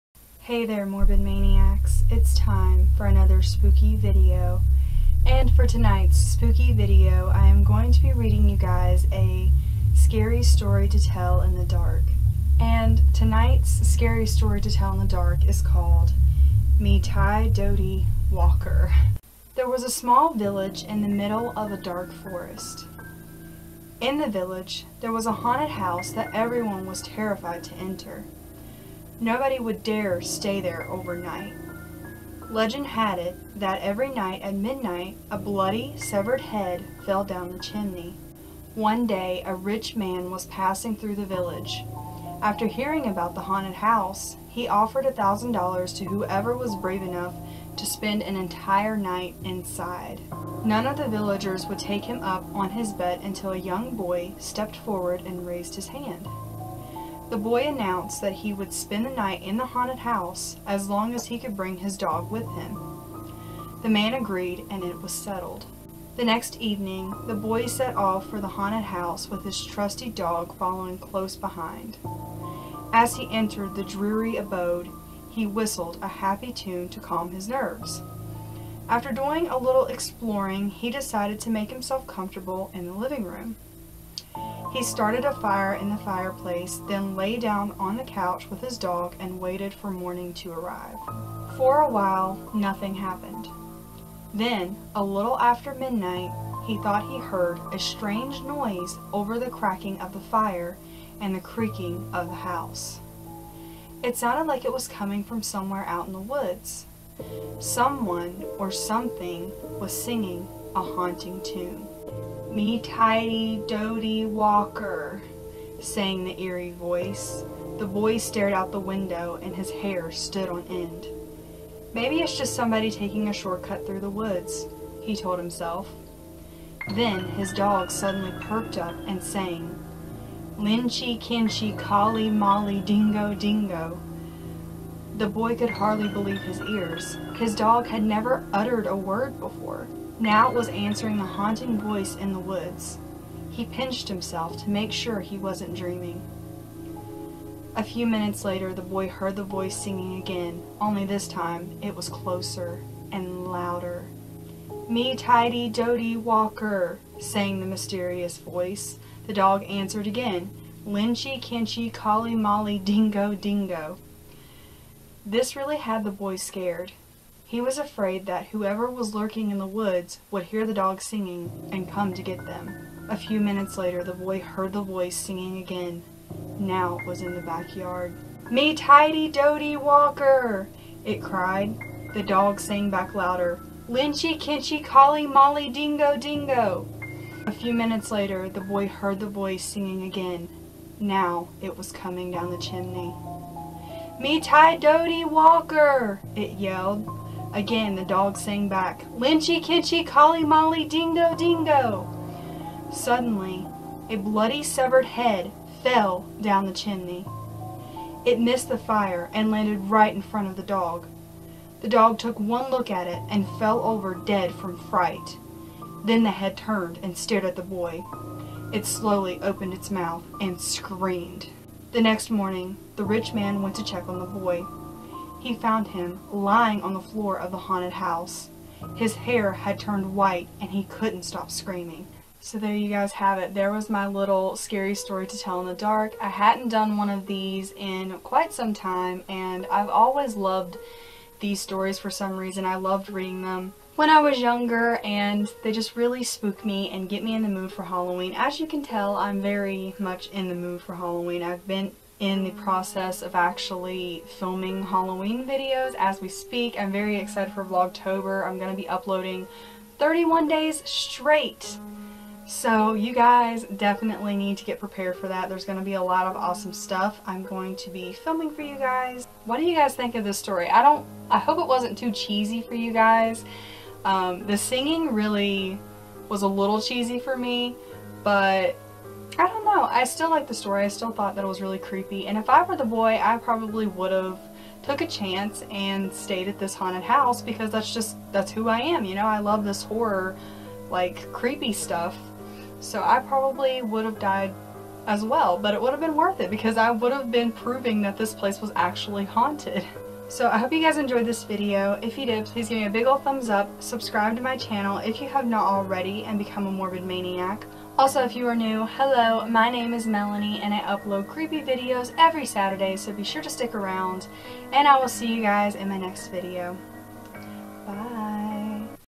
ME TIE DOUGHTY WALKER | Scary Stories To Tell In The Dark
Me Tie Doughty Walker is a scary story about a young boy who stays at a haunted house and has a scary encounter with a severed head.

By: Myuuji 🎵

If you're new to my channel, my name is Melanie. 😉